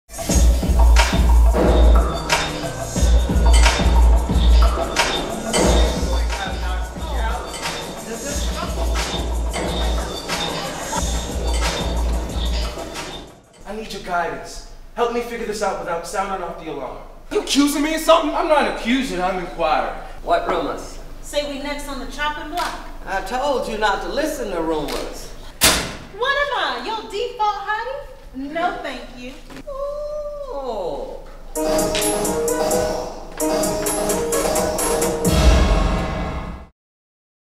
Skeleton Crew by Dominique Morisseau
The future is thrown into uncertainty when rumors seep through a Detroit auto plant at the start of the Great Recession. A makeshift family of workers swap stories, share dreams and make tough choices. Dominique Morisseau, a noted new voice in the American theater, draws comparison to Lorraine Hansberry, August Wilson, and Arthur Miller.